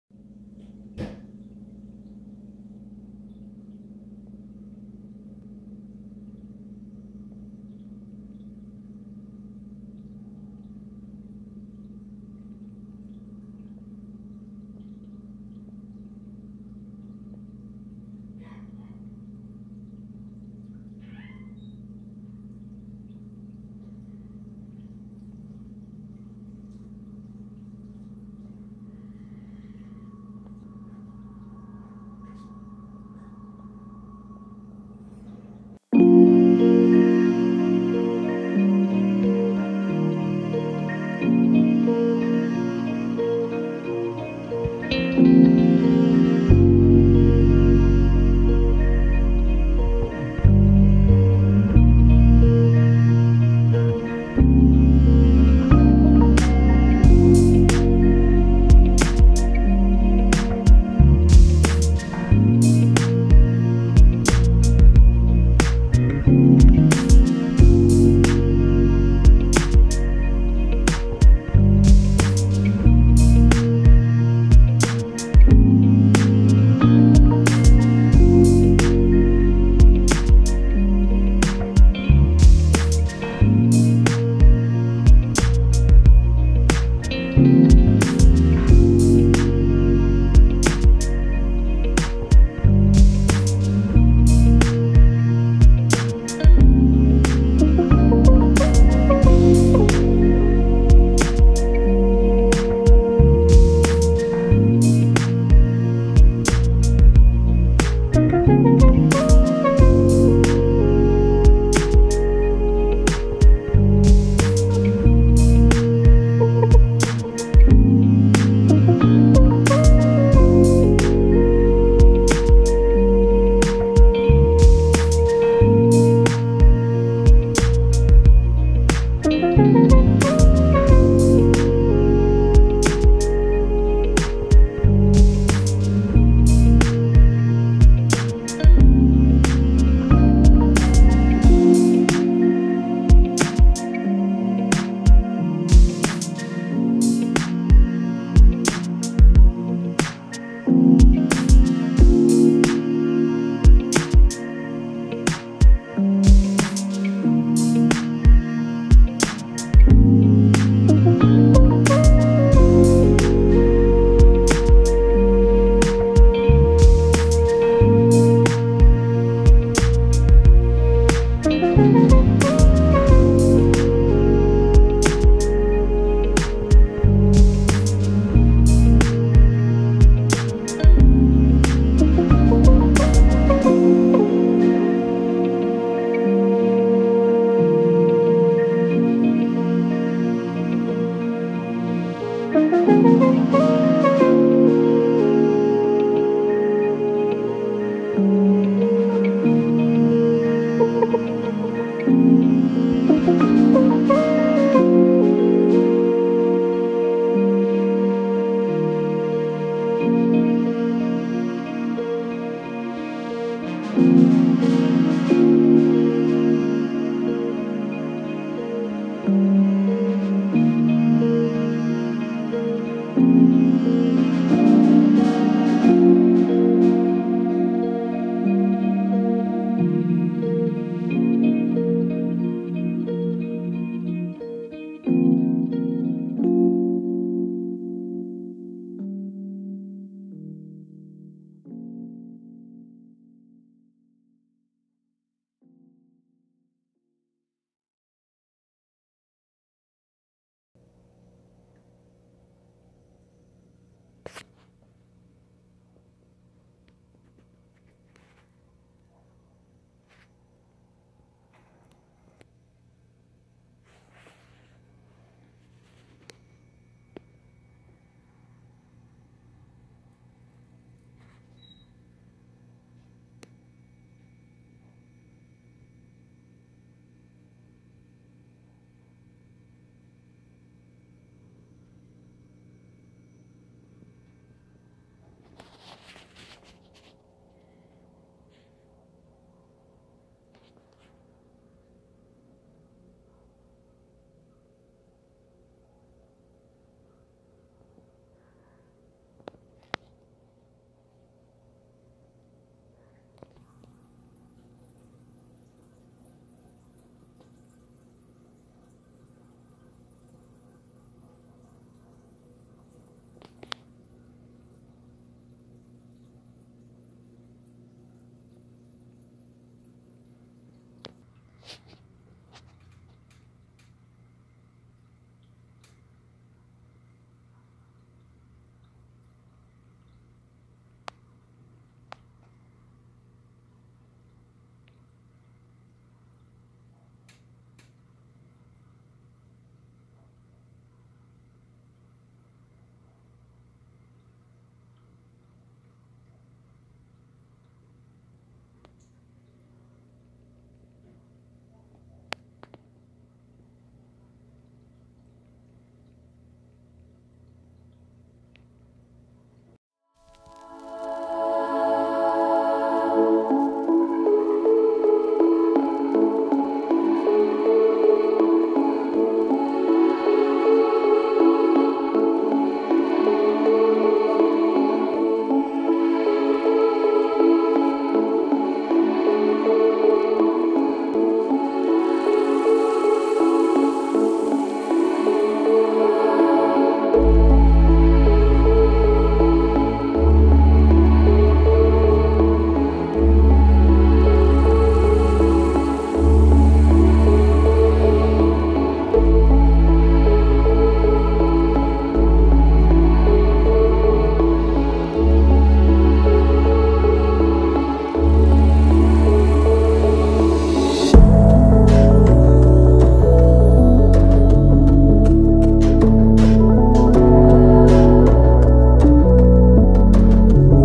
Mr. Sealand Tv Last update
Donated the fish and corals. Sold the tank and everything with it. I'm still into the hobby and later on I will invest in a larger aquarium when the time is right.